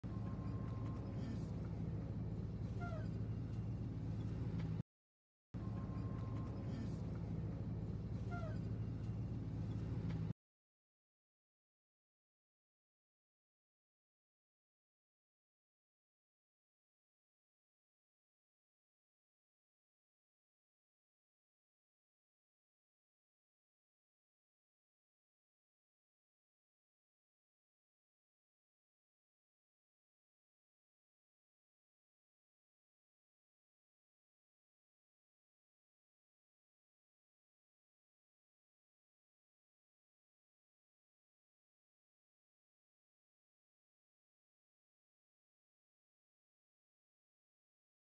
California car plunges off cliff
A car in California can be seen driving off a cliff along a highway.